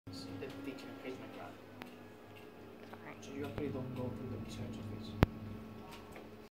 Scanning the Thylacine at ANU
Using a Rangevision scanner.